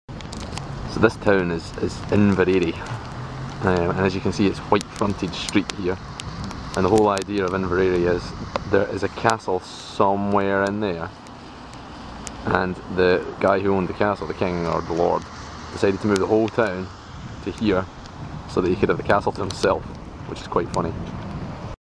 Inveraray Town (STS 050)
Inveraray Town in Argyll faces onto the Loch Fyne shores. It's a beautiful place and very peaceful despite the sheer number of tourists around. The town was moved to the present location from being around the nearby castle in the past. The new location is much nicer mind you.